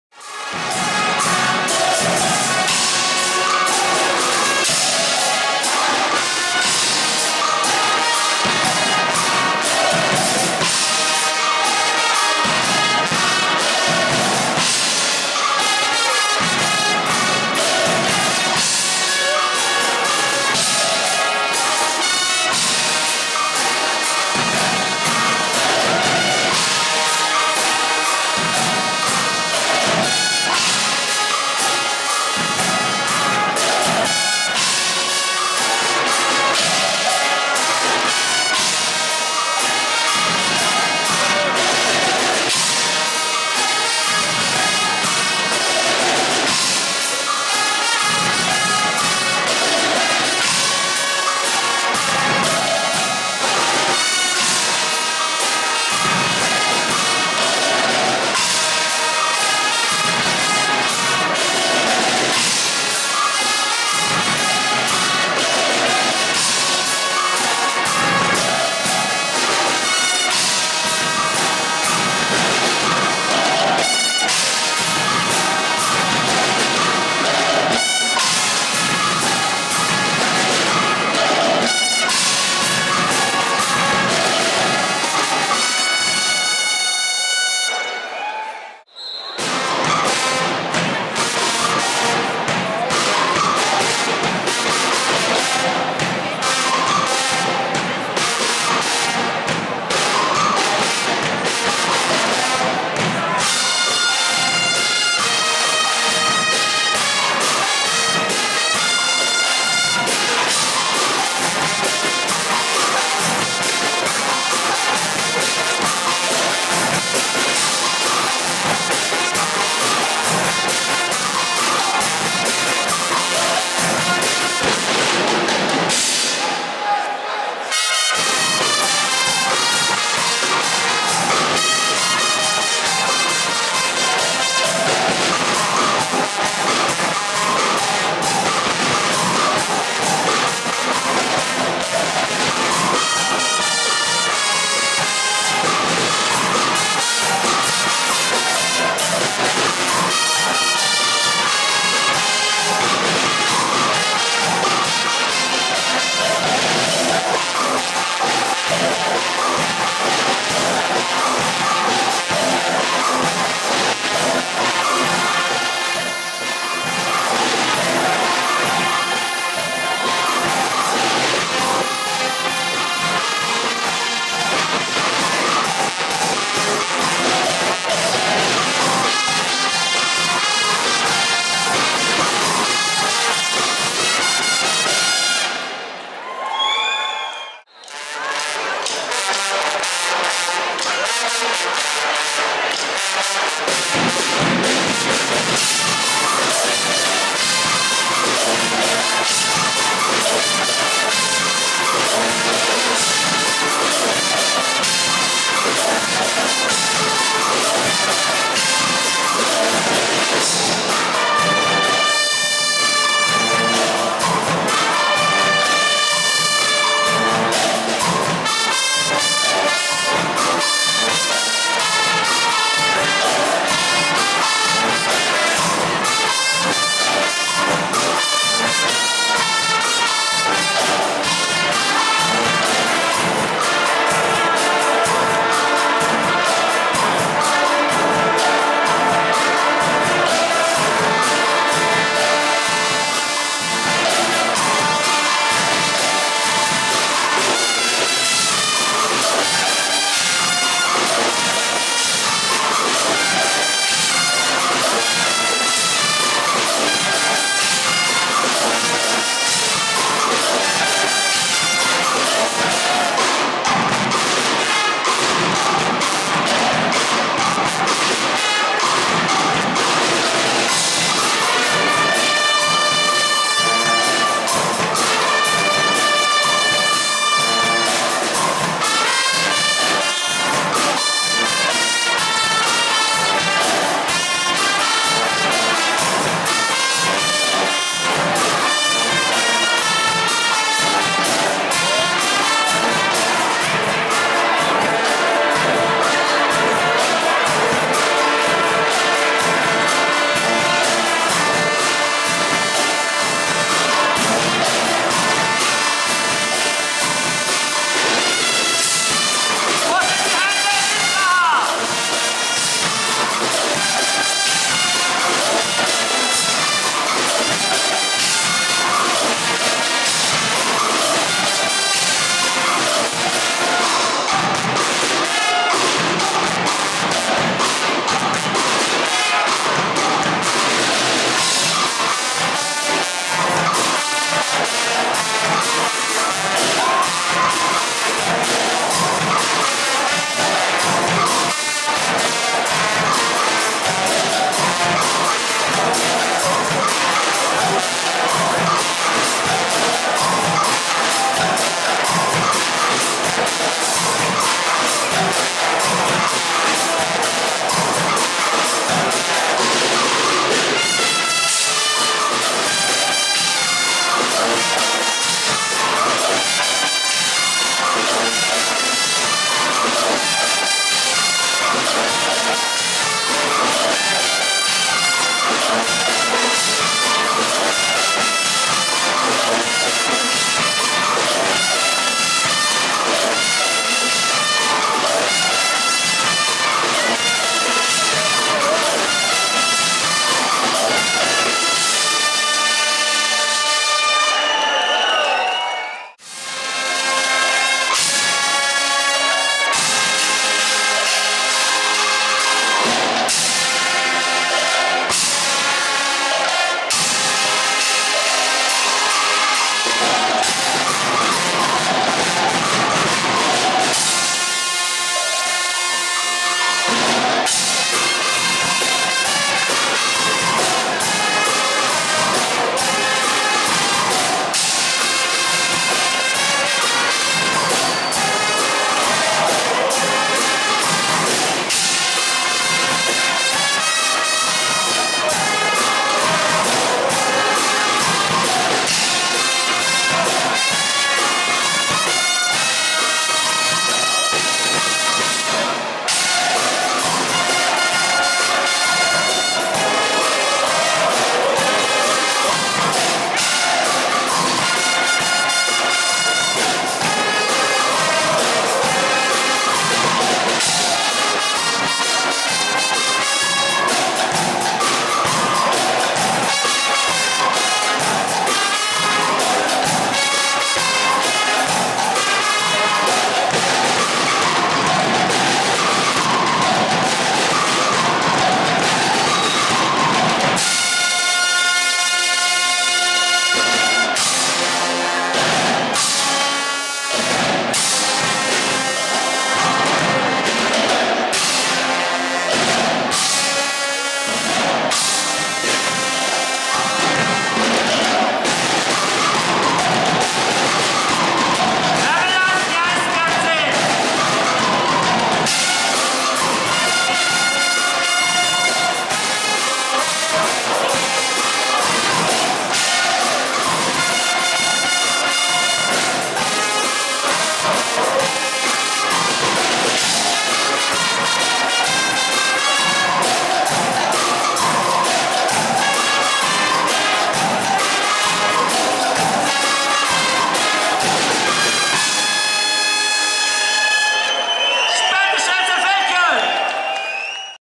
23. int. Monsterkonzert Spältaschränzer Feldkirch 2017
Unter dem Motto "Könige und Bettler" präsentierten sich die Spältaschränzer Feldkirch am Monster. Unser Stammgäste werden heuer zum ersten Mal von Stefan Fuchs dirigiert.